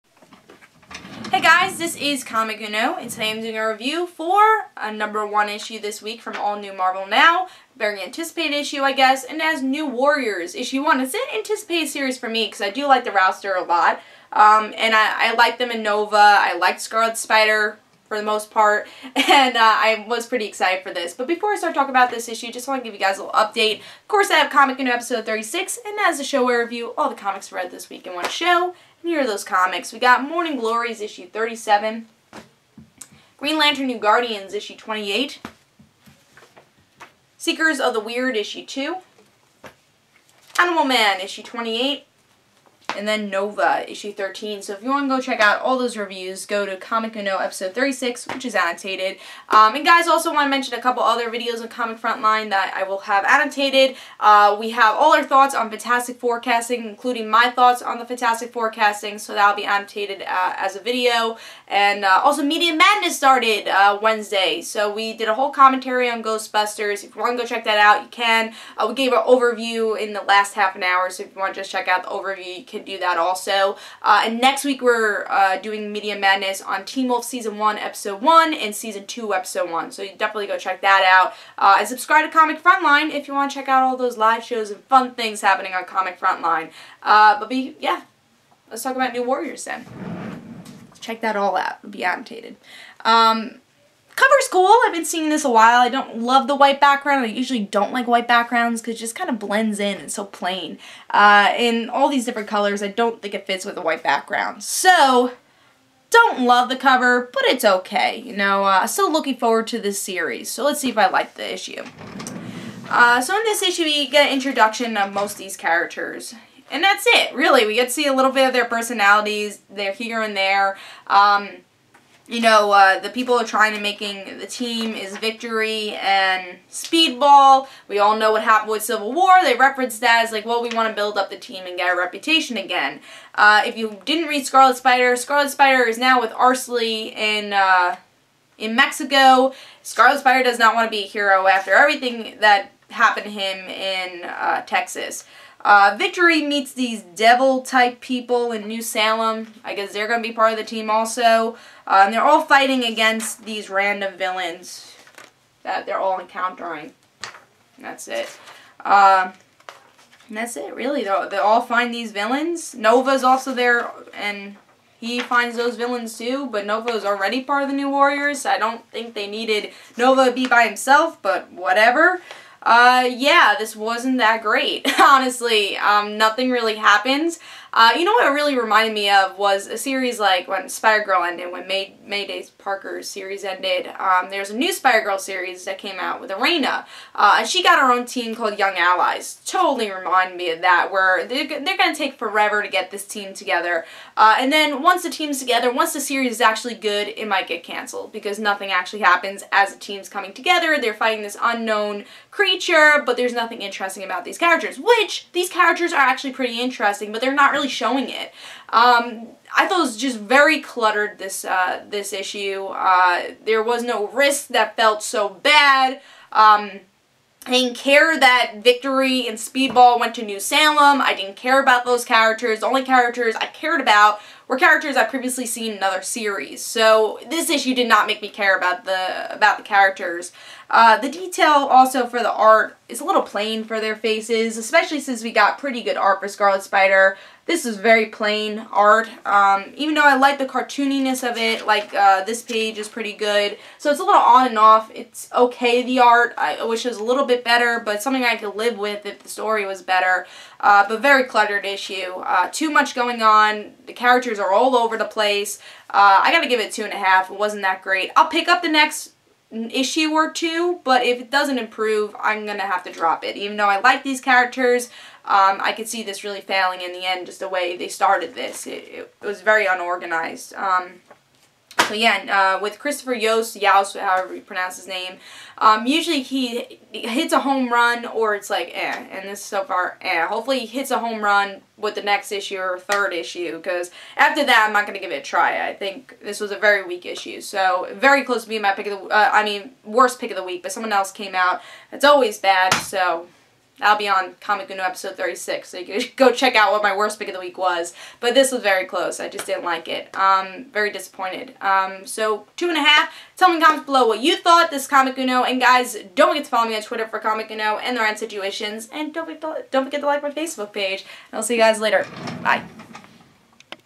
Comic Uno New Warriors #1 (review)
Here is my comic book review for New Warriors issue 1.